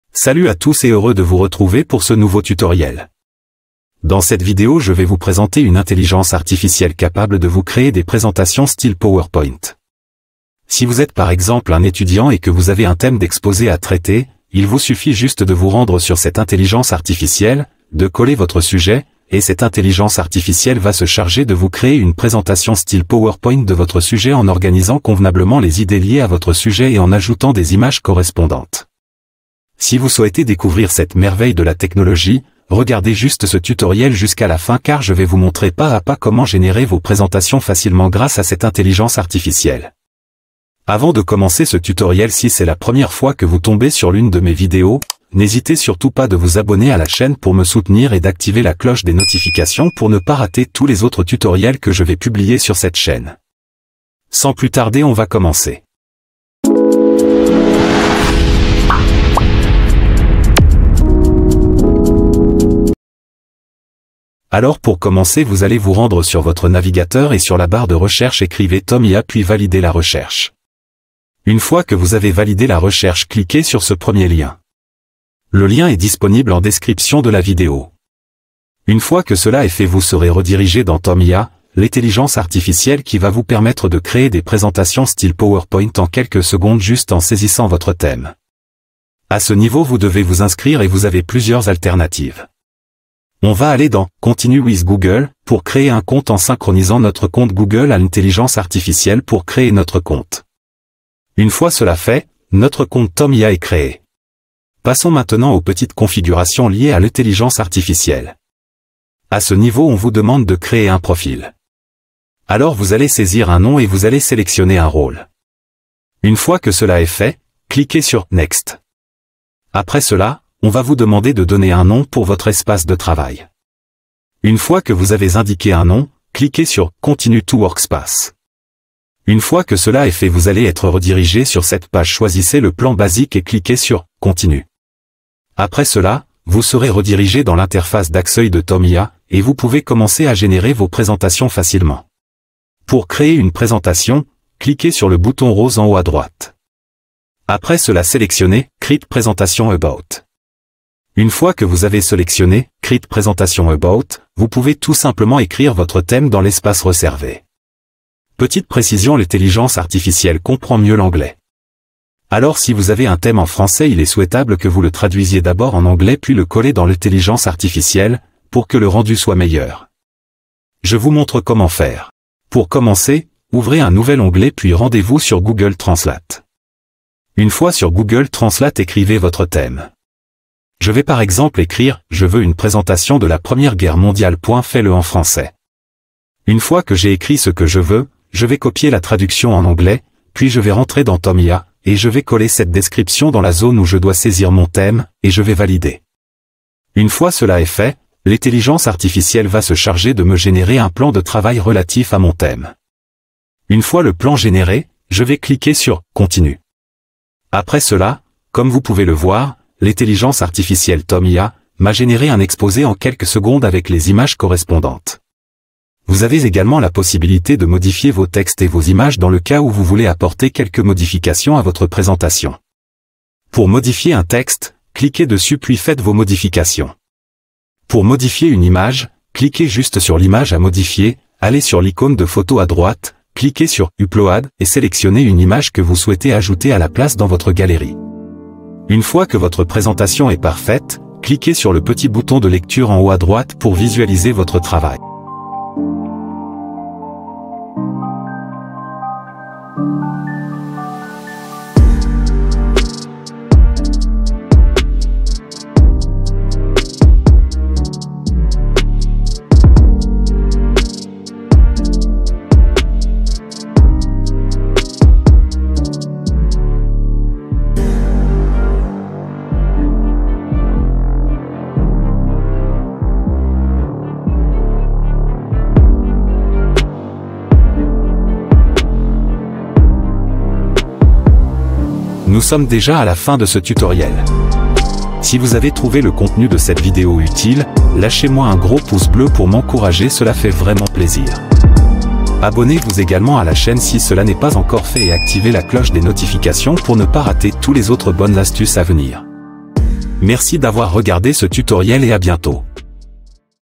INCROYABLE!! Voici l'IA qui peut générer une présentation style POWERPOINT en deux clics.
Si tu cherches une IA capable de créer tes Présentations style POWERPOINT, ce Tutoriel est fait pour toi. Dans ce tutoriel je présente Tome ia, L'intelligence artificielle capable de te générer une présentation en quelques secondes grace à ton sujet d'exposé. Si tu veux profiter de cet outil regarde ce tuto jusqu'à la fin. Bon visionnage.

LIENS UTILES:
Lien Ali express pour vos achats en ligne